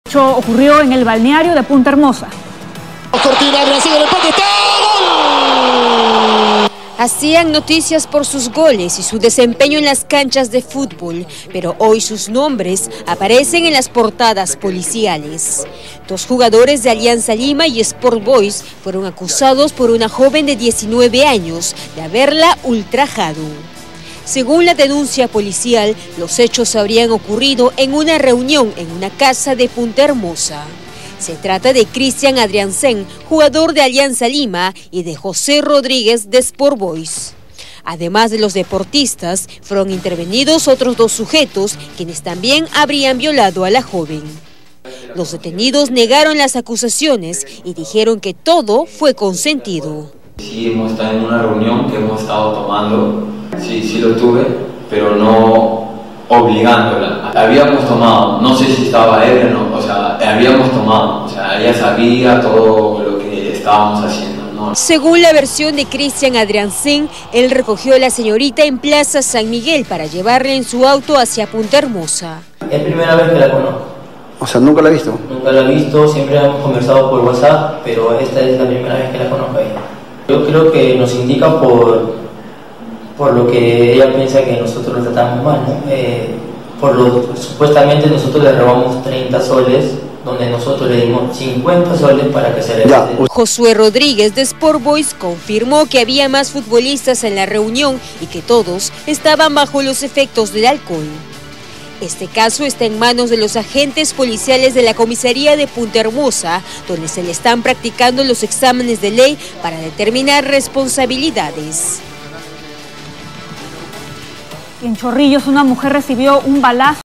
Acusan a futbolistas de violar a una joven en Punta Hermosa (2/2)
Dos jugadores de Alianza Lima y Sport Boys fueron acusados por una joven de 19 años de haberla ultrajado sexualmente. Según la denuncia policial, los hechos habrían ocurrido en una reunión en una casa de Punta Hermosa.

Christian Adrianzén, jugador de Alianza Lima, y Josué Rodríguez, del club Sport Boys, fueron intervenidos junto a otros dos sujetos, quienes también habrían violado a la joven. Ellos negaron que se tratara de una violación y dijeron que todo fue consentido.

Según la versión de Adrianzén, recogió a la señorita en Plaza San Miguel para llevarla en su auto hacia Punta Hermosa. Rodríguez confirmó que había más futbolistas en la reunión y que todos estaban bajo los efectos del alcohol.

El caso está en manos de los agentes policiales de la comisaria de Punta Hermosa.